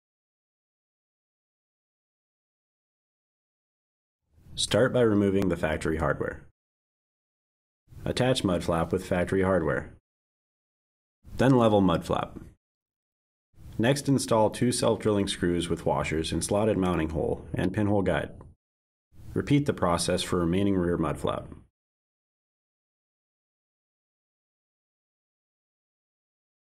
2009-18 RAM 1500, 19-24 Classic & 10-19 2500/3500 - Rear ROCKSTAR™ Splash Guard Install Video
Watch how quickly you can install ROCKSTAR™ Splash Guard Mud Flaps on the rear of your 2009-18 RAM 1500, Classic or 2010-19 RAM 2500/3500 pickup. The tough rubber protects your trailer, boat and other toys from potentially hazardous road debris. Easily add protection and style to your Dodge RAM truck with these model specific, behind the wheel mud flaps featuring precision cut mounting holes. Outfit your ROCKSTAR™ Splash Guard Mud Flaps with stainless steel trim plates or go without for an all black streamlined appearance.